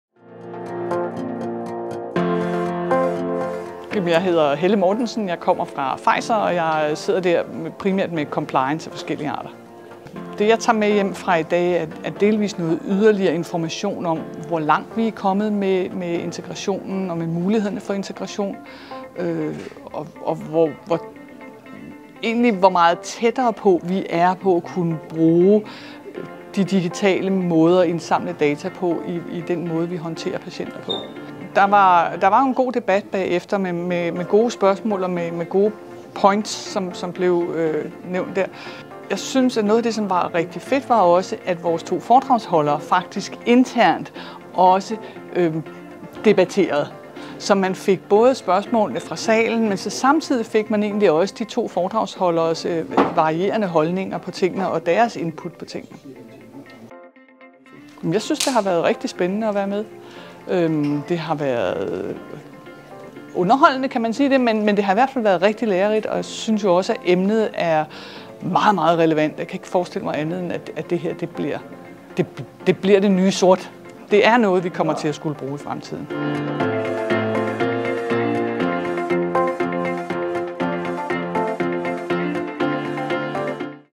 Life Science Morning Briefing 13. april 2023
Future Health: Beyond the pill - når lægemidler møder det digitale univers.